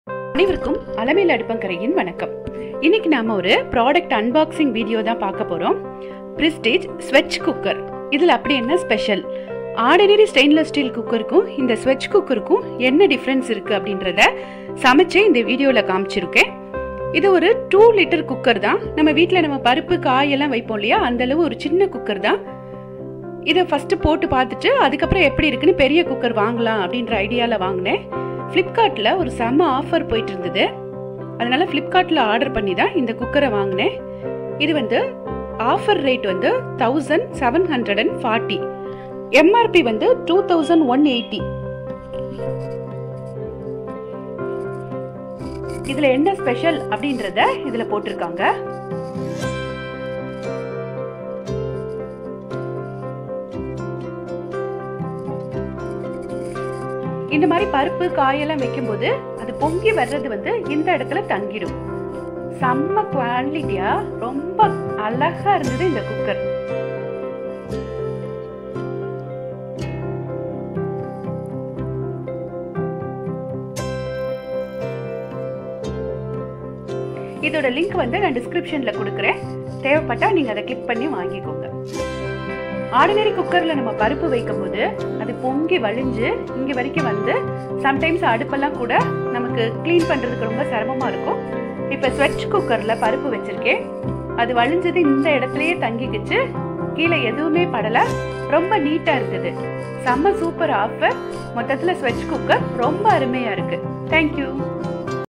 Prestige Svachh Induction Bottom Pressure Cooker - Review in Tamil
Prestige Svachh Cooker Review:
Unboxing and Review of Prestige Svachh Cooker are given in our video. What is the Specialities of Svachh Cooker, Difference between oddinary cooker and Svachh cooker is explained in our video. Svach cooker is Purchased in Flipkart Big Billion Offers.
Prestige Svachh Induction Bottom Pressure Cooker :
Prestige Svachh Deluxe Alpha 2 L Induction Bottom Pressure Cooker